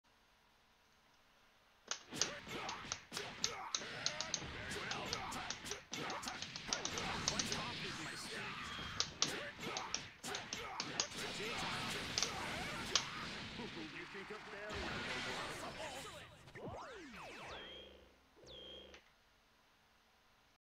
Cell- 100% corner Carry 6k dmg
2m, 5m, jc, jMLL2H, SD, j LL, jc, j LLLS, 2M, VK, 2m, m, j LL, jc, jLL, 236L, 236L+M

Idea from swagmountains on reddit